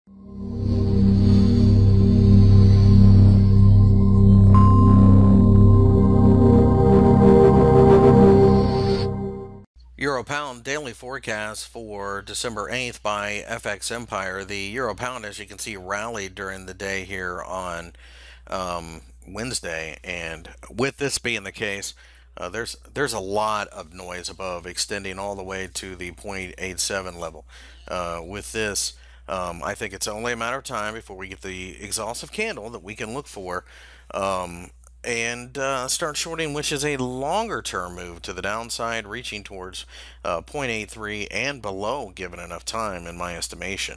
EUR/GBP Technical Analysis for December 08 2016 by FXEmpire.com
- December 08 2016 currency daily technical analysis for the EUR/GBP pair. ), Forex Technical Analysis ), Forex Fundamental Analysis ) and Forex Brokers ) on FX Empire